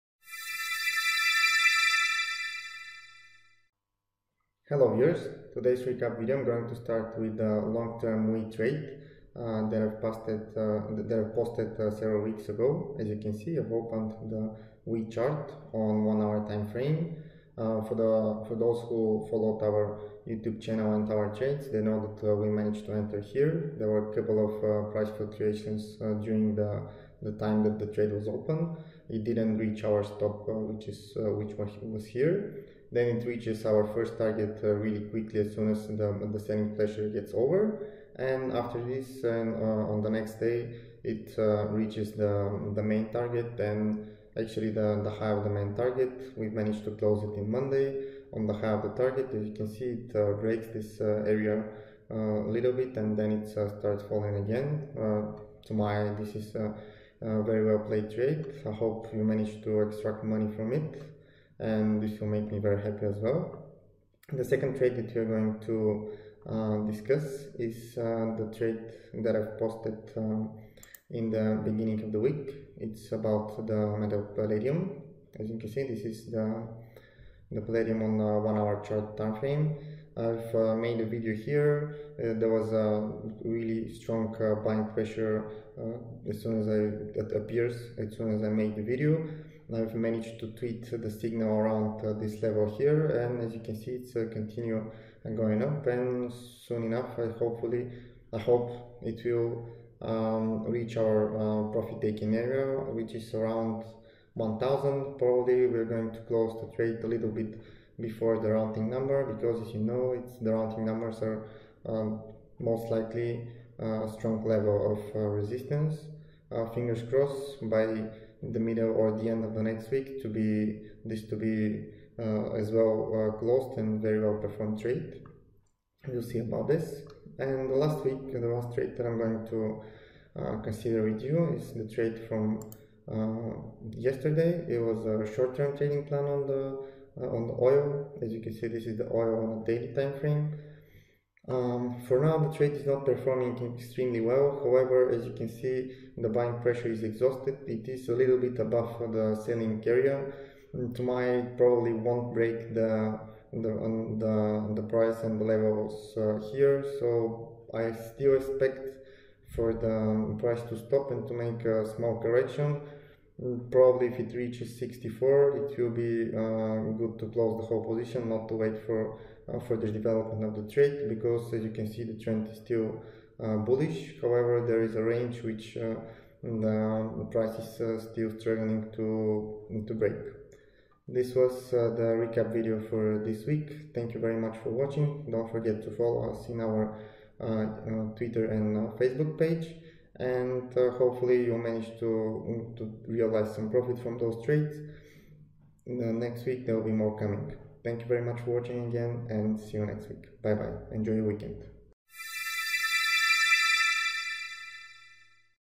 Weekly Commodities Recap
The last several trades which are involved in today's recap video performed really well we hope to keep that momentum for the future and to provide you with more profitable trade ideas. Enjoy your weekend.

Последните няколко трейда се развиха много добре и се надяваме да задържим това представяне и за напред и да ви предоставим още печеливши трейд идеи. Насладете се на слънчевият уикенд.

Отказ от отговорност - Това видео не е предложение за търговия, а просто анализ. Всякакъв капитал използван от ваша страна за търговия е на ваш риск.